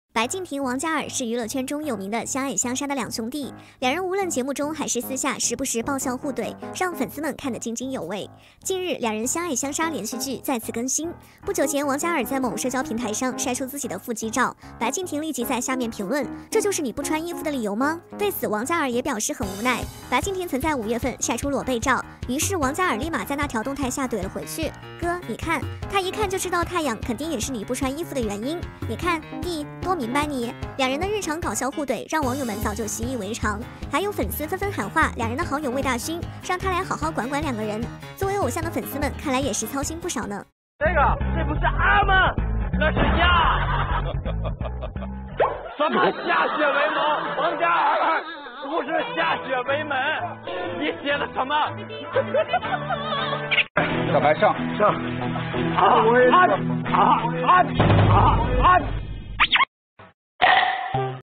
王嘉尔白敬亭爆笑互怼，网友：魏大勋该来管管了
简介王嘉尔和白敬亭是娱乐圈的好兄弟，近日王嘉尔发了一张没穿上衣的照片，二人就开始了互怼模式，王嘉尔直接回怼白敬亭的裸背照，网友纷纷喊话让魏大勋来管管两兄弟